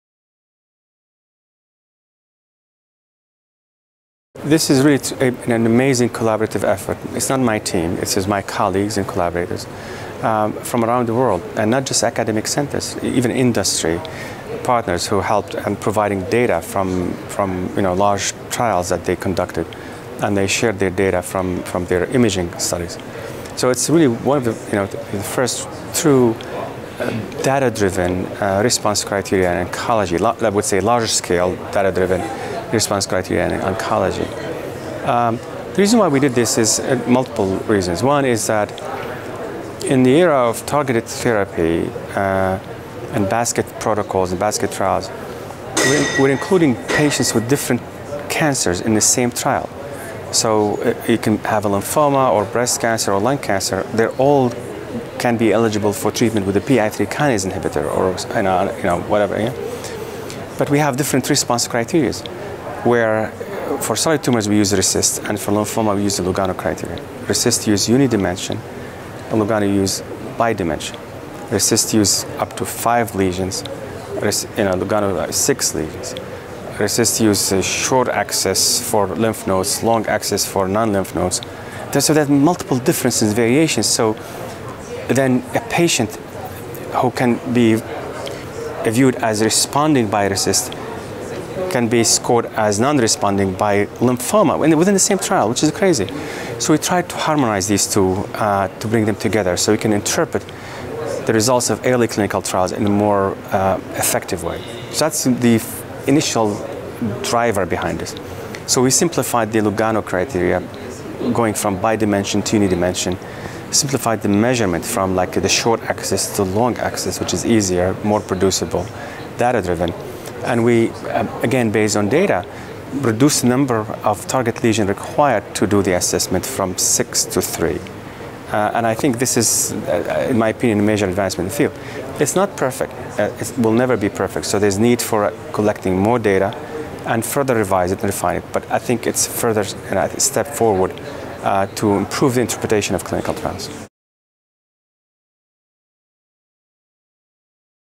RECIST: Harmonizing response criteria in oncology
Anas Younes, MD, from the Memorial Sloane-Kettering Cancer Center, New York, NY, talks to us about harmonising the criteria for evaluating response in lymphoma clinical trials with the Response Evaluation Criteria in Solid Tumors (RECIST). A group of international lymphoma experts from academic centers and pharmaceutical companies, radiologists, and statisticians have collaborated to harmonize lymphoma response criteria with RECIST, hypothesizing that single-dimension measurement may be used to assess response with results similar to those with standard criteria, as detailed by Prof. Younes. This interview was filmed at the American Society of Oncology (ASCO) 2017 Annual Meeting in Chicago, IL.